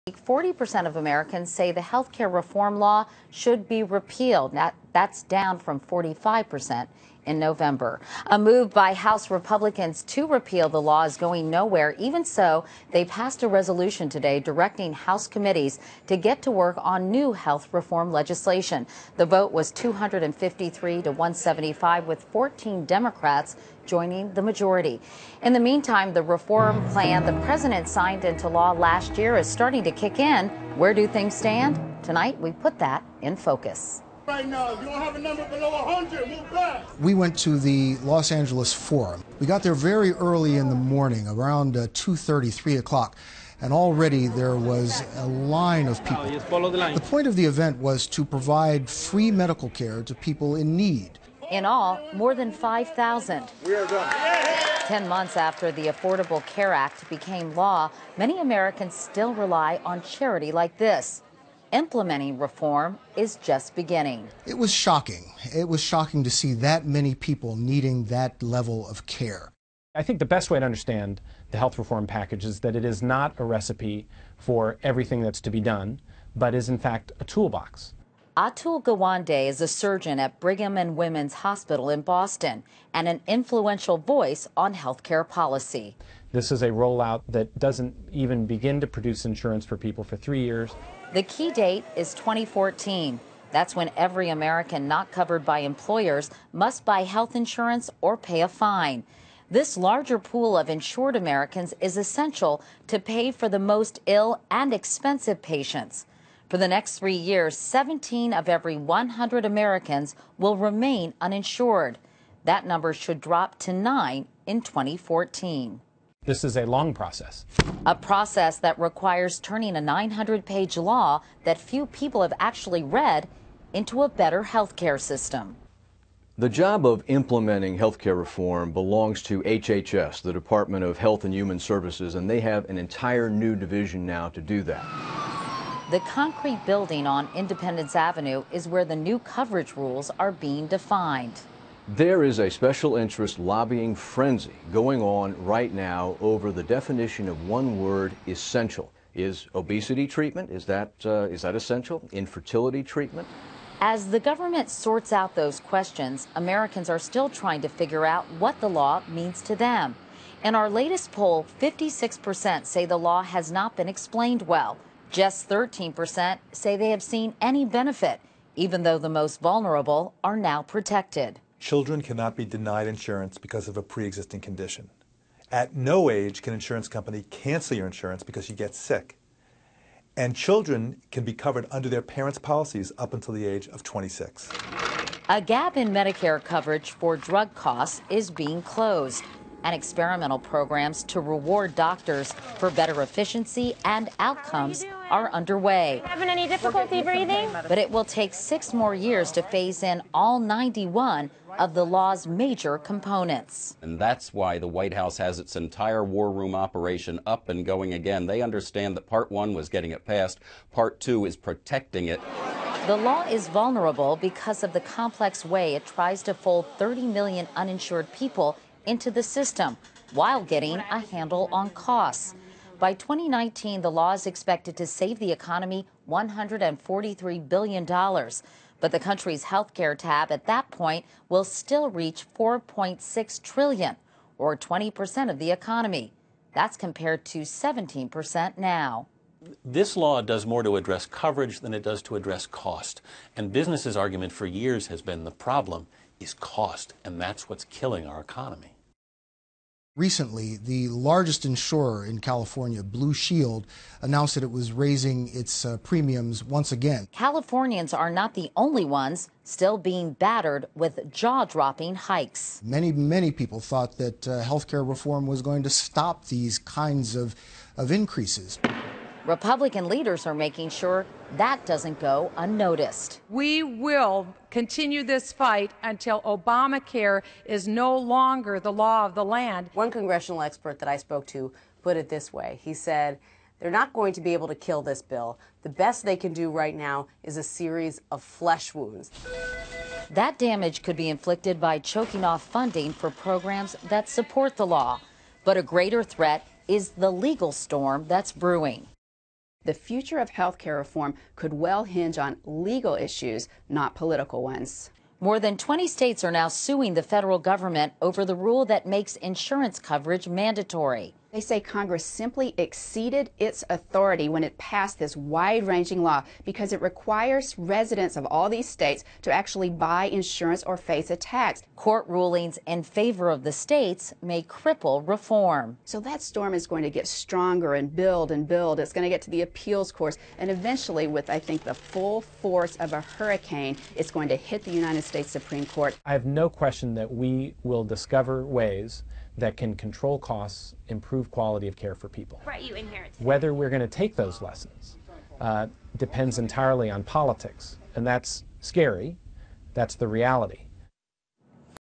How ObamaCare Is Working
A look into how The American Healthcare Act is working.

0:00 - Repeal Efforts Go Nowhere
0:27 - Reforms Beginning
1:09 - The Toolbox
2:06 - Independence Avenue
3:33 - Savings Vs. Costs
4:08 - Flesh Wounds
4:58 - The Future